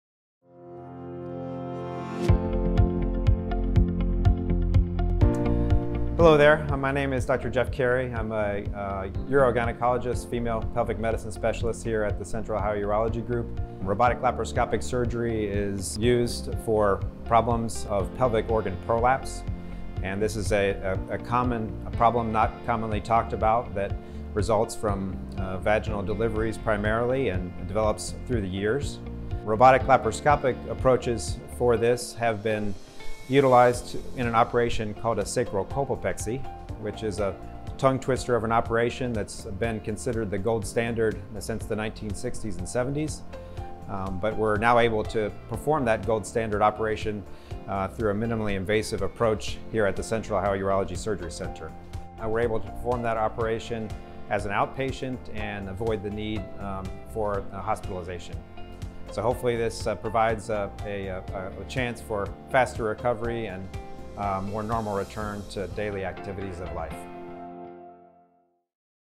Dr. Jeffrey Carey, Urogynecologist specialist, discusses robotic laparoscopic surgery.
Dr. Jeffrey Carey, Urogynecologist specialist, discusses robotic laparoscopic surgery for pelvic organ prolapse. Robotic laparoscopic surgery is used for problems with pelvic organ prolapse, a common problem for women which primarily results from vaginal deliveries. Robotic surgery allows a minimally invasive approach for this operation as an outpatient surgery with no hospitalization.